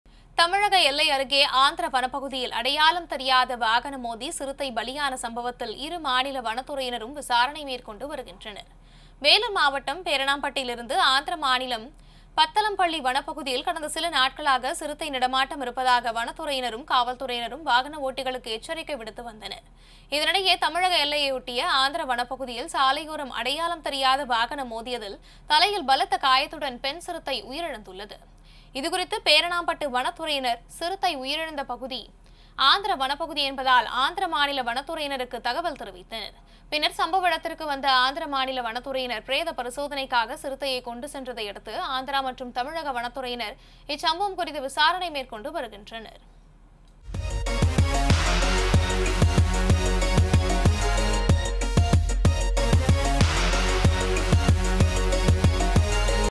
Leopard | Hit and Run | அடையாளம் தெரியாத வாகனம் மோதி சிறுத்தை பலி!
தமிழக எல்லை அருகே ஆந்திர வனப்பகுதியில் அடையாளம் தெரியாத வாகனம் மோதி சிறுத்தை பலியான சம்பவத்தில்,  இரு மாநில வனத்துறையினரும் விசாரணை மேற்கொண்டு வருகின்றனர்.